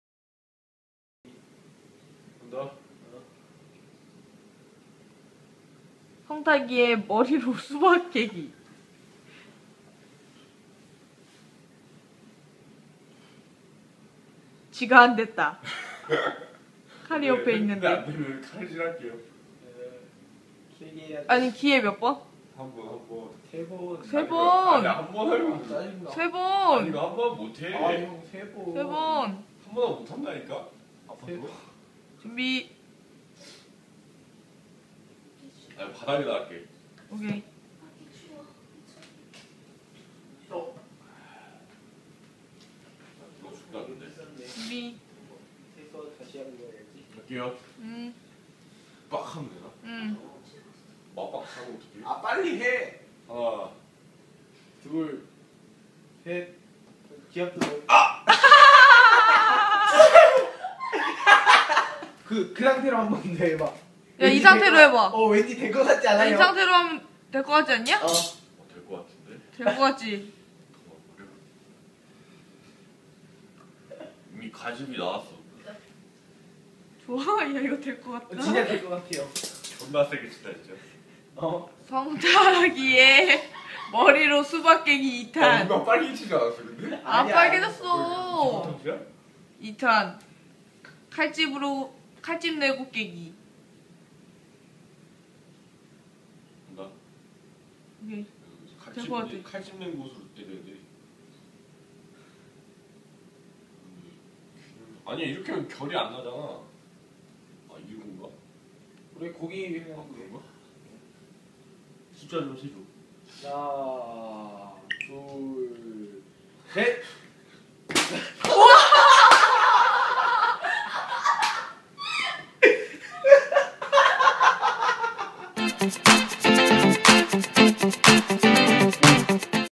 양띵TV옴므[옴므의 억지추억여행 머리로 수박깨기 도전!!]
ㅋㅋㅋㅋㅋㅋㅋㅋㅋㅋㅋㅋㅋㅋㅋㅋㅋㅋㅋㅋㅋㅋㅋㅋㅋㅋㅋㅋㅋㅋㅋㅋ...

*양띵TV 멤버들의 개인 유튜브 채널은 아래 링크를 클릭하세요.